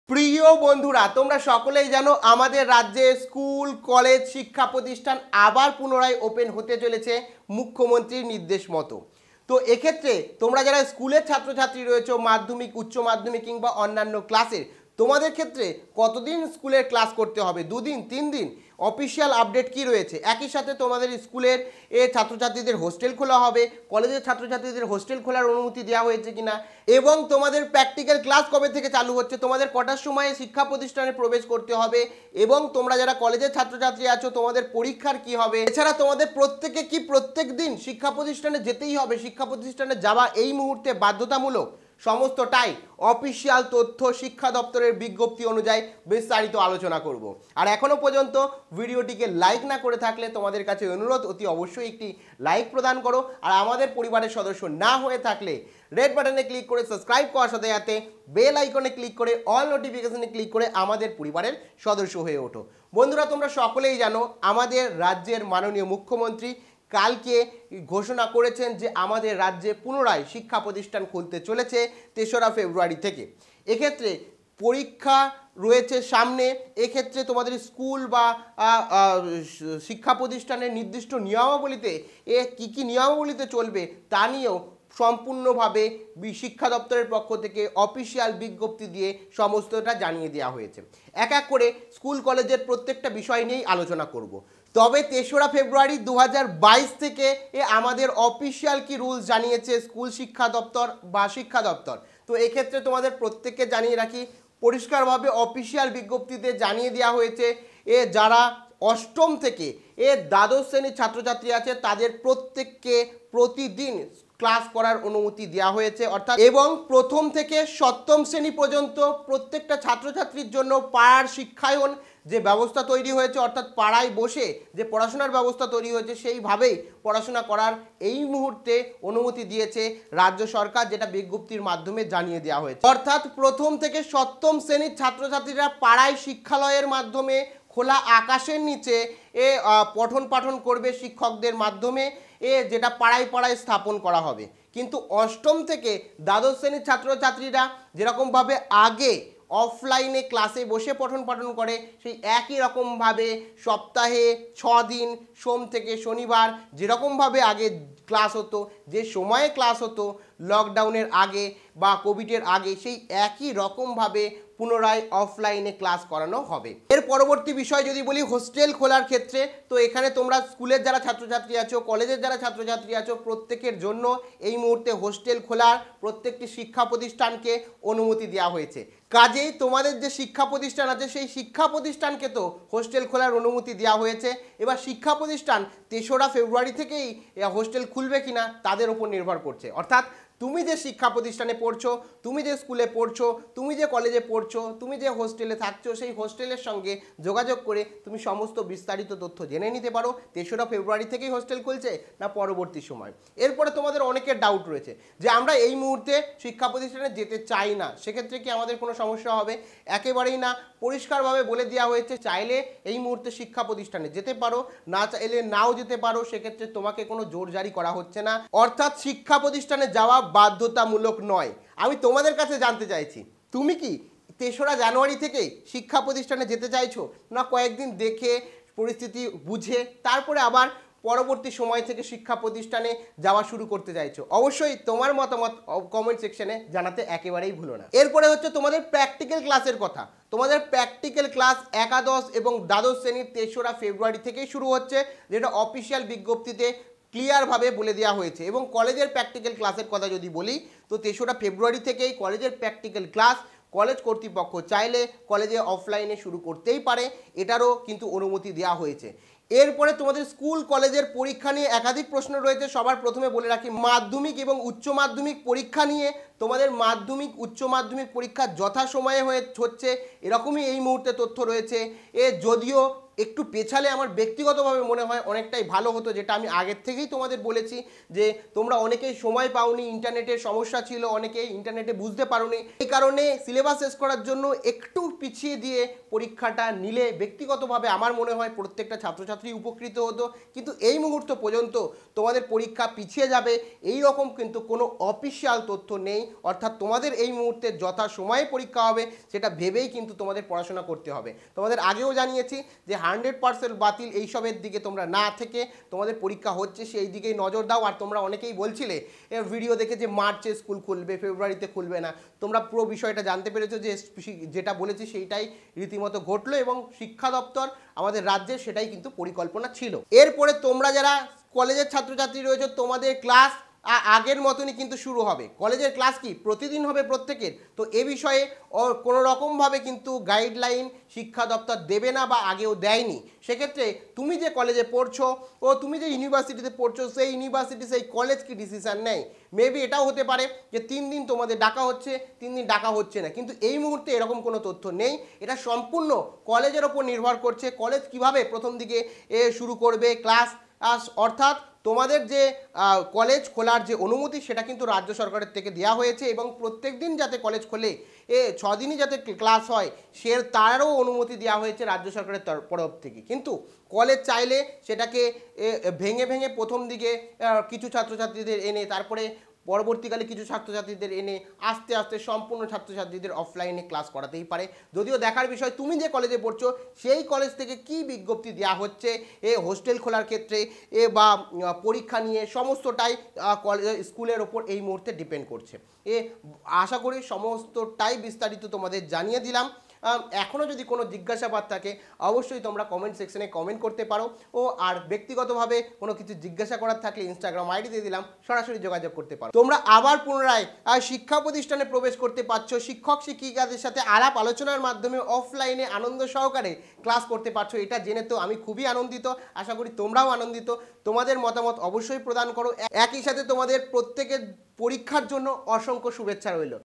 স্কুল-কলেজের ক্লাস ও পরীক্ষা : WB school college University open 2022 , hostel open, practical exam
শিক্ষা দপ্তর ও স্কুল শিক্ষা দপ্তরের অফিশিয়াল বিজ্ঞপ্তিতে ছাত্র-ছাত্রীদের হোস্টেল , প্র্যাকটিক্যাল ক্লাস, শিক্ষা প্রতিষ্ঠানে যাওয়া বাধ্যতামূলক , কখন যাবে শিক্ষাপ্রতিষ্ঠানে , এবং এই মুহুর্তের পরীক্ষা নিয়ে সমস্ত তথ্য ভিডিওতে প্রেরণ করেছি
।WB school college University open by Mamata Banerjee

merit list
 1st year malda college pass course and honours merit